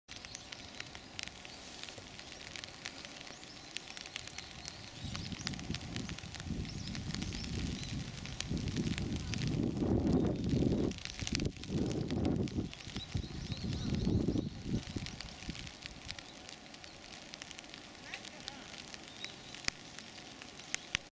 三春 福聚寺のしだれ桜 満開！ 00862
大きなシダレザクラは、400年ほど前に滝桜から植え継いだものだという。境内には三春の城下町を築いた田村氏三代の墓もある。